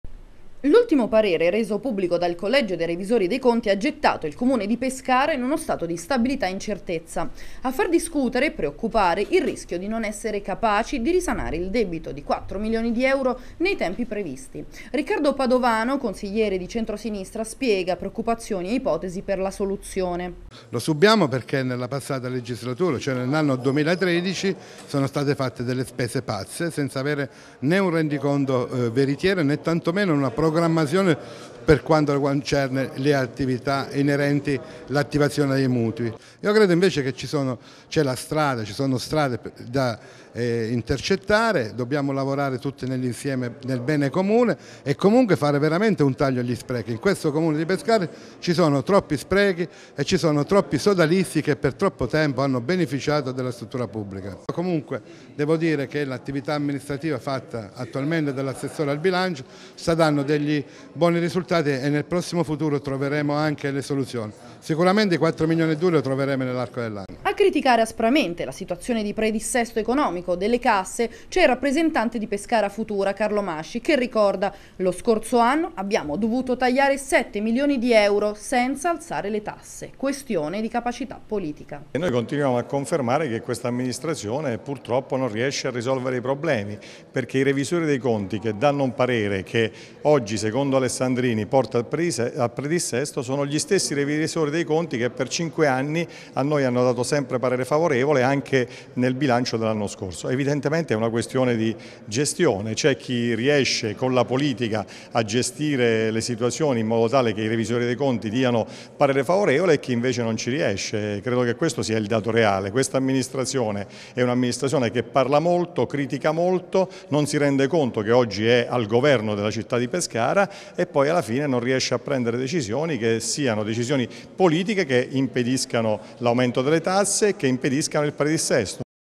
Tasse Pescara. Scontro tra maggioranza e opposizione
Scontro tra maggioranza e opposizione sui conti del comune di Pescara. Per il centrodestra la maggioranza non fa altro che aumentare le tasse, la replica del centrosinistra che invita alla ricerca della soluzione migliore